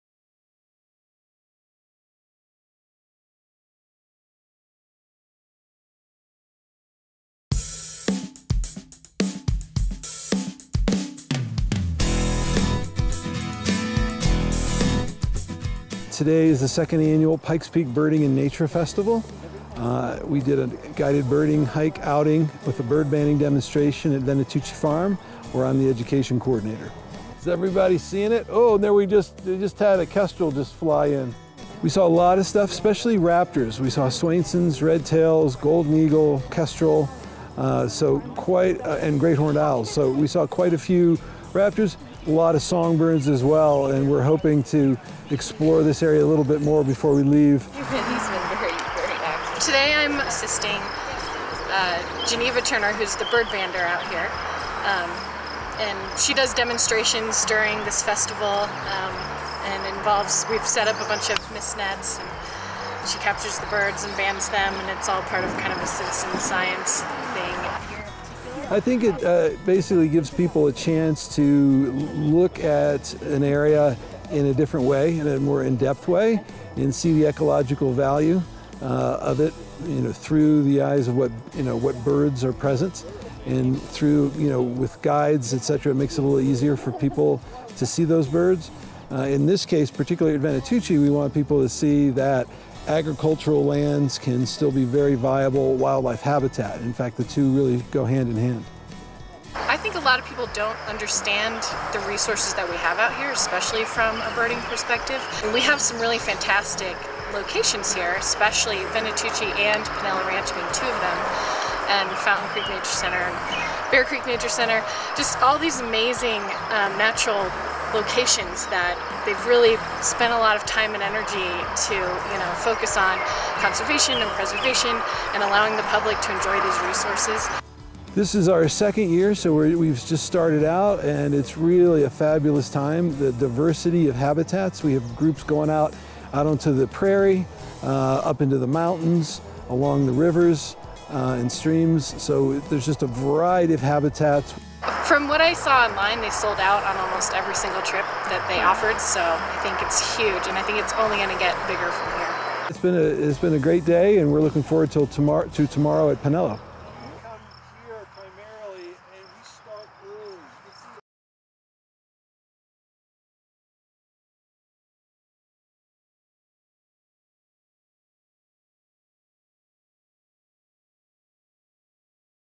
2016 Pikes Peak Nature & Birding Festival - Venetucci Farms Birding Hike
Attendees of the 2nd Annual 2016 Pikes Peak Nature & Birding Festival attend the Saturday, May 21, 2016 morning event at Venetucci Farms, where they explore the grounds and spot the wide variety of bird species on the premises.